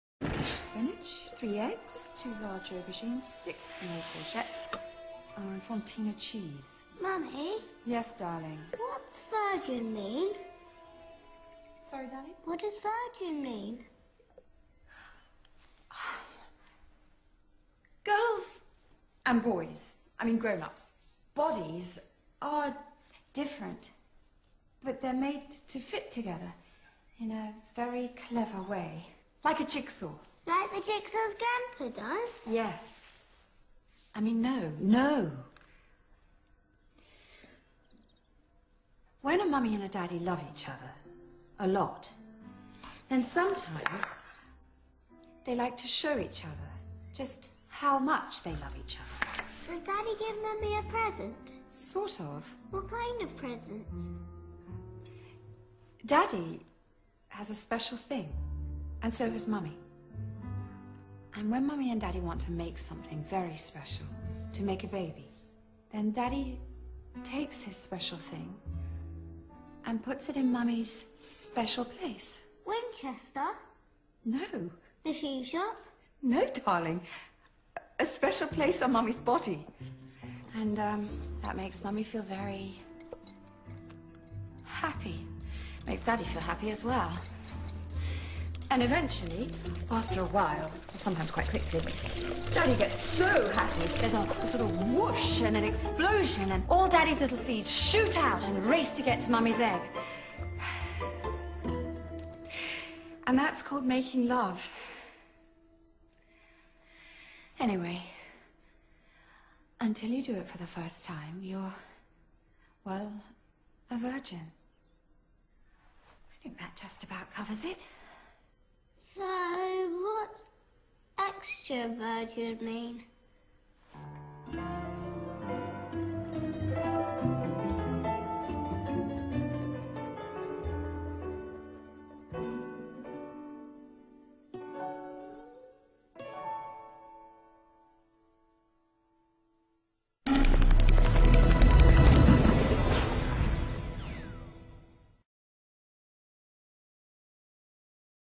Reclama la ulei extraVIRGIN
La fel ca şi restul reclamelor, aceasta face parte din sporirea VÎNZĂRILOR! Aici specialiştii au vrut să luminze pînă şi mintea unui copil prin intemediul unei mame "înflăcărate" în explicaţii.